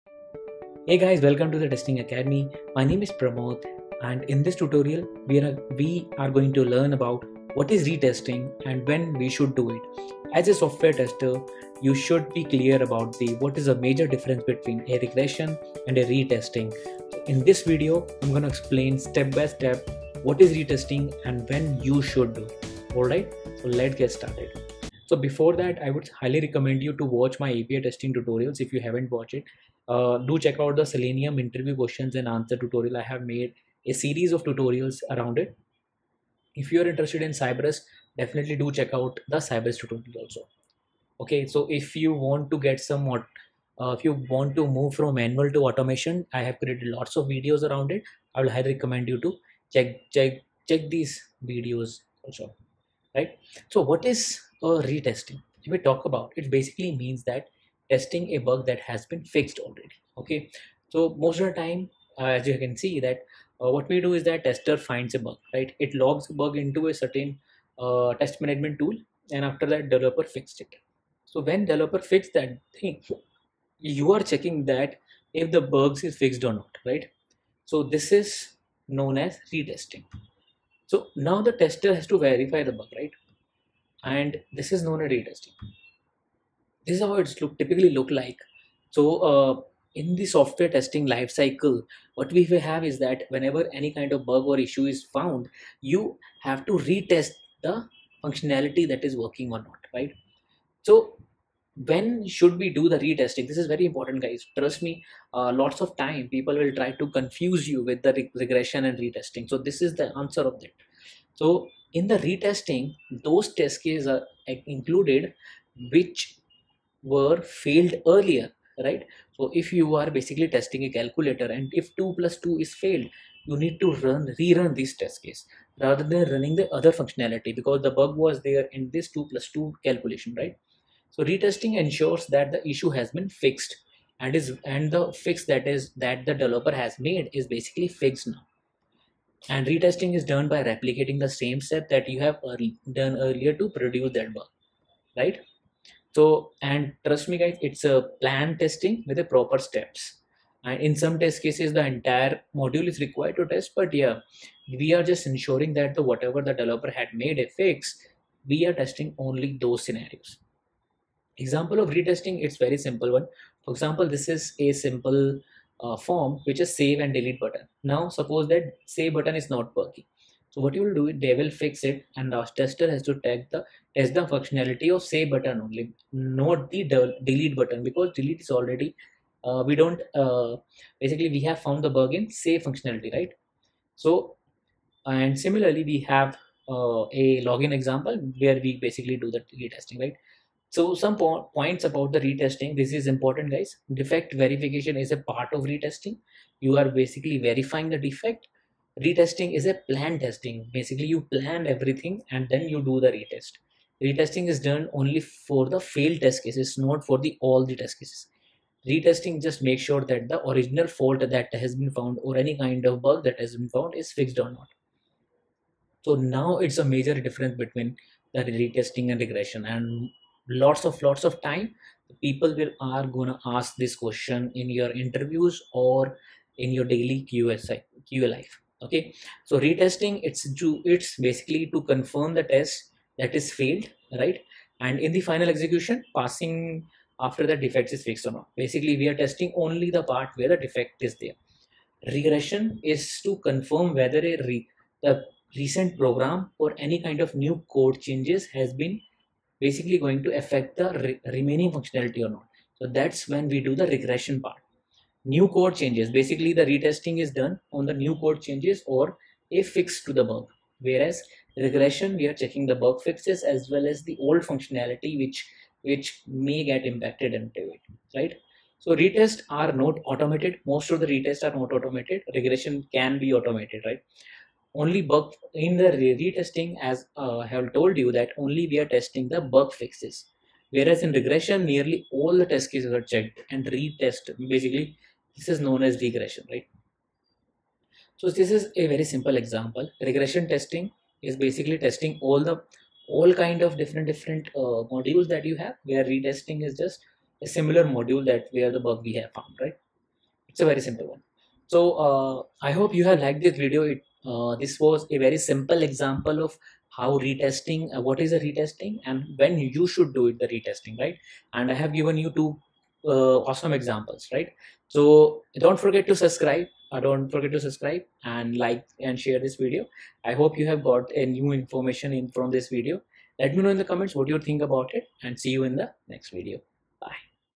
Retesting vs Regression Testing : When We Do Retesting in Software Development?(With Example)
In this video of Retesting vs Regression Testing, We are going to learn about the What is the main difference between two retesting and regression.

What is Retesting?

In the retesting in software testing, It basically means testing a bug that has been fixed.
Most of the Time Tester finds a bug in the Application(Web or mobile), It logs it in the JIRA and Assign it to Dev, after dev fixes it, Dev assign back to Tester to verification.
Now Tester verify the bug, This is known the
Retesting.

Benefits of retesting
1. Test the cases that are failed only
2. Fast execution.
3. Only have to perform the important or failed cases

When, ReTesting is Done?
I
n retesting those test cases are included which were failed earlier.
Retesting ensures that the issue has been fixed and is working as expected.
Retesting is done by replicating the same scenario with same data in new build.
It is a planned testing with proper steps of verification
In some cases the entire module is required to be re-tested to ensure the quality of the module.

Example of Retesting
- Suppose Save button is not working
- Dev fixed it
- Now Tester have to Check the Functionality again.
This is Retesting

I have added retesting testing example and please note that
Defect verification is the part of re-testing
Re-testing is a planned testing
Retesting is done only for failed test cases
Re-testing makes sure that the original fault has been corrected

Retesting vs Regression

Retesting :
To confirm the test cases that failed in the final execution are passing after the defects are fixed.
Retest are not automated.
Only bug fixes related test-cases are checked.

Regression :
To confirm whether a recent program or code change has not adversely affected existing features.
Check the bug fixes.
Regression can be automated.
Nearly all the testcases are checked in Regression.
New code changes should not have any side effects to existing functionalities

retesting definition